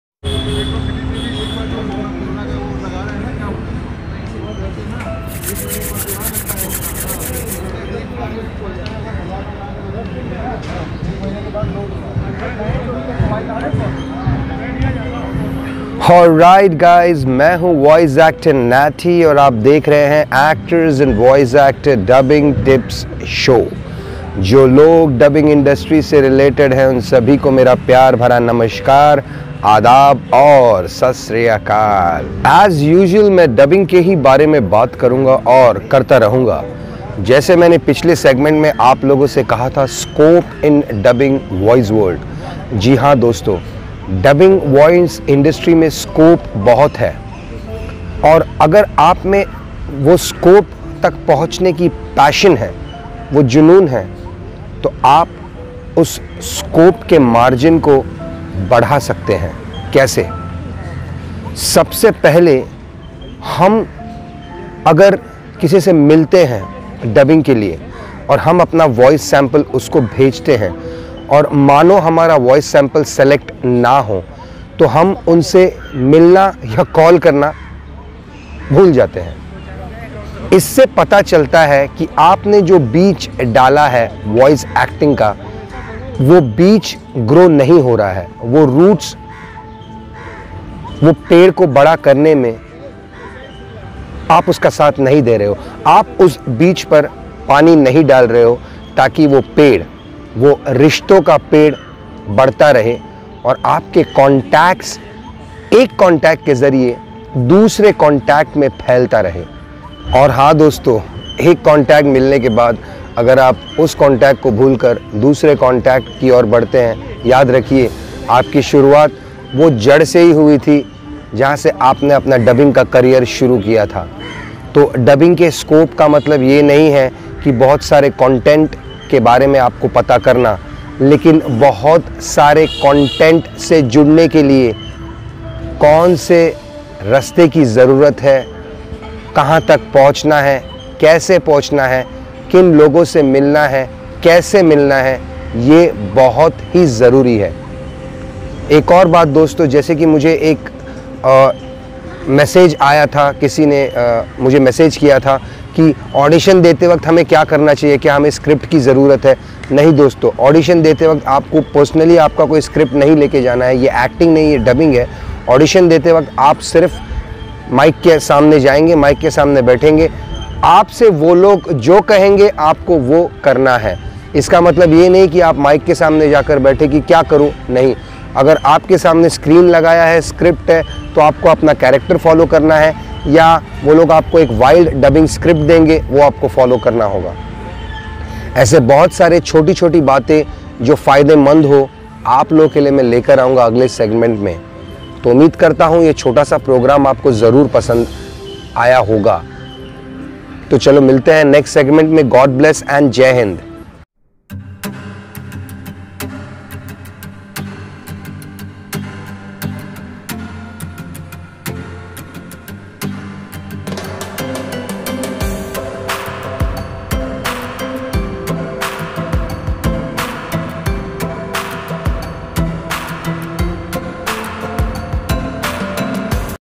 Scope In Dubbing Industry
It's a  Huge Scope in Dubbing Industry  , If We Are Entering Voice World Most Important Thing Is Contact, You should Remember The Start of Your Career How You Were So Humble Towards Your Work.